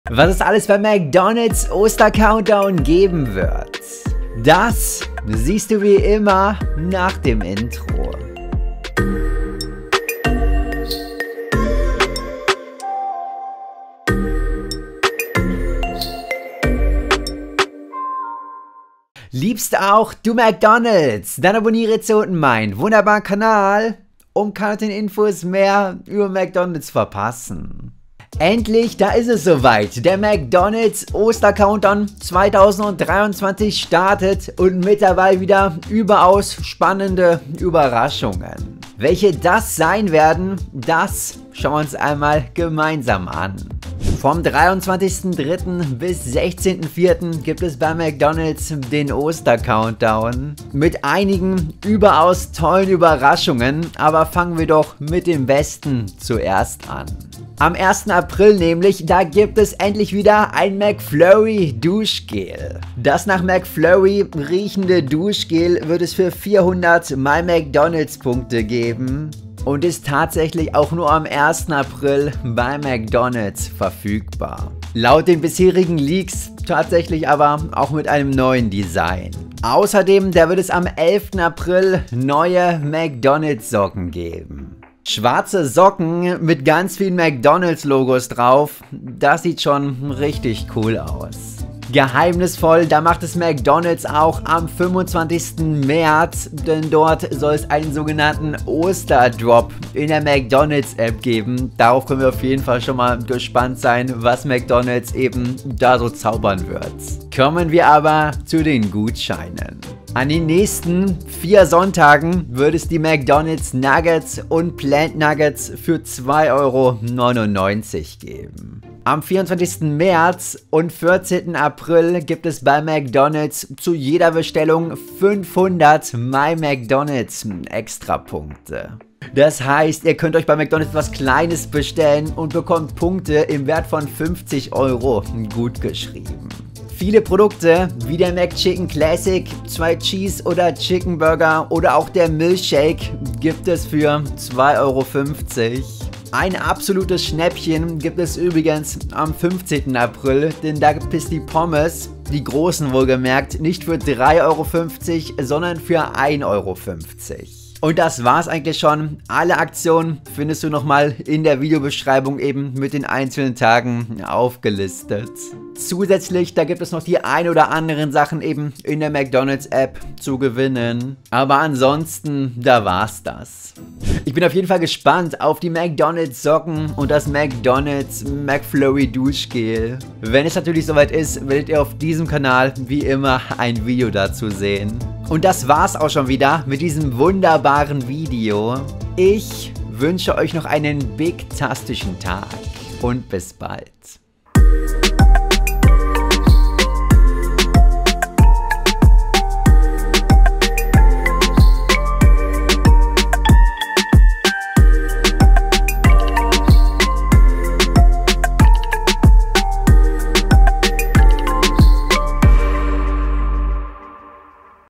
McDonalds - Ostercountdown 2023 - Alle Aktionen Leak
Der McDonalds Ostercountdown 2023 ist zurück.

Alle Produkte:
Do. 23.03. - McChicken Classic für 2,50€
Fr. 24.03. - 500 Extra MyMcDonalds Punkte
Sa. 25.03. - Osterdrop in der App
So. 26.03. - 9 McNuggets für 2,99€

Mo. 27.03. - Coca Cola Lemon Gratis ⭐
Di. 28.03. - McChicken Classic für 2,50€
Mi. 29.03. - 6 McNuggets für 2,00€
Do. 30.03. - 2 Klassiker für 6,00€
Fr. 31.03. - Milchshake 2,50€
Sa. 01.04. - McFlurry Duschgel ⭐
So. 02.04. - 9 McNuggets für 2,99€

Mo. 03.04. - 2 Cheese- o. Chickenburger 2,50€
Di. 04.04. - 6 McNuggets für 2,00€
Mi. 05.04. - McChicken Classic 2,50€
Do. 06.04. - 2 Klassiker für 6,00€
Fr. 07.04. - Filet-o-Fish 2,00€
Sa. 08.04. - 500 Extra MyMcDonalds Punkte
So. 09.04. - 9 McNuggets für 2,99€

Mo. 10.04. - Milchshake 0,4l für 2,50€
Di. 11.04. - McMenü Small für 6,99€ + Gratis Socken ⭐
Mi. 12.04. - McChicken Classic 2,50€
Do. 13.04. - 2 Cheese- o. Chickenburger 2,50€
Fr. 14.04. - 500 Extra MyMcDonalds Punkte
Sa. 15.04. - Pommes groß 1,50€
So. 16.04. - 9 McNuggets für 2,99€

▸ 🍓 TIMESTAMPS :
00:00 : Anfang
00:27 : Einführung
00:57 : Mcflurry Duschgel
01:20 : McDonalds Socken
01:32 : Osterdrop in der App
01:47 : Gutscheine
02:45 : Sonstiges
02:54 : Ende
03:13 : Outro